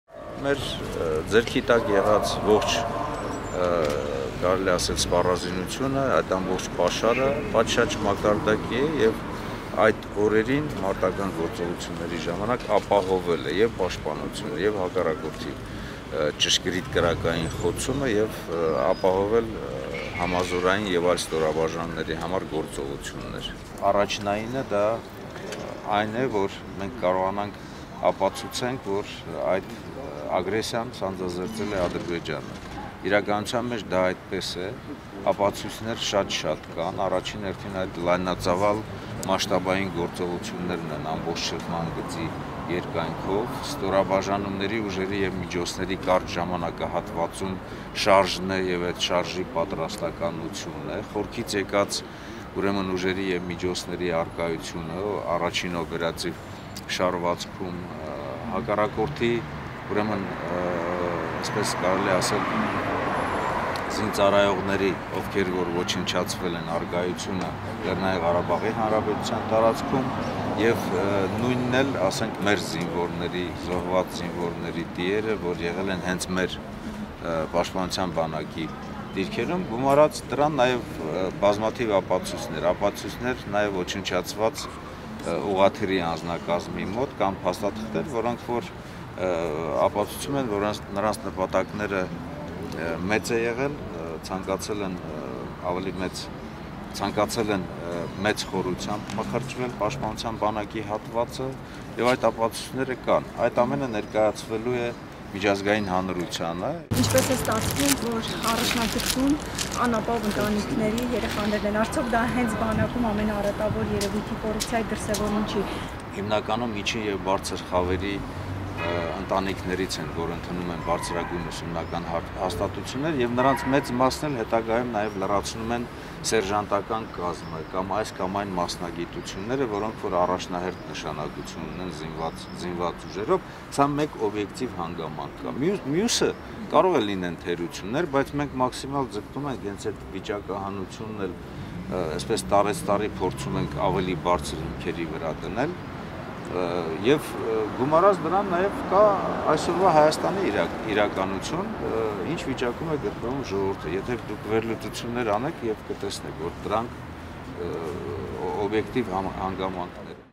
Օհանյան. «Առաջնային է ապացուցել, որ ագրեսիան սանձազերծել է Ադրբեջանը»
Հայաստանի պաշտպանության նախարար Սեյրան Օհանյանը պատասխանել է լրագրողների հարցերին։